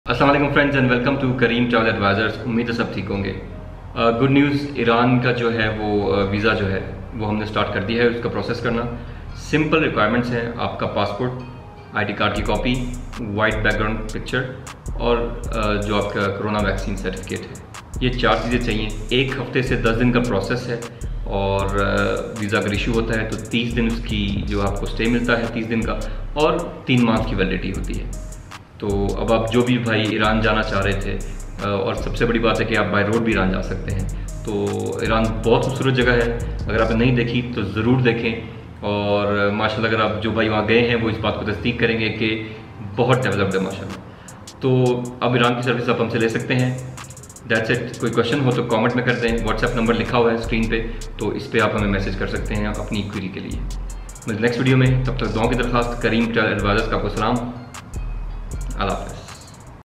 Iran Visa Update | Iran by Road bhi ja saktay hen
Ab hum iran k visa ki services bhi de rahe hen.